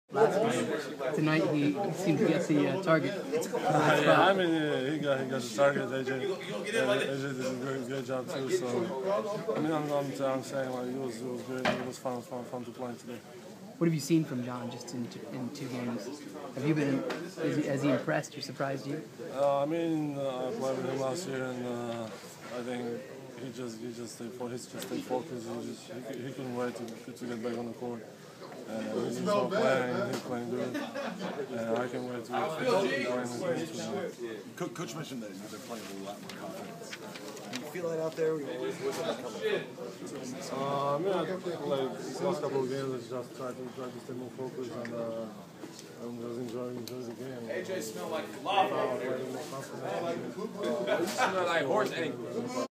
Jan Vesely Post-game Wiz Magic 1-14-13
Jan Vesely talking about his performance off the bench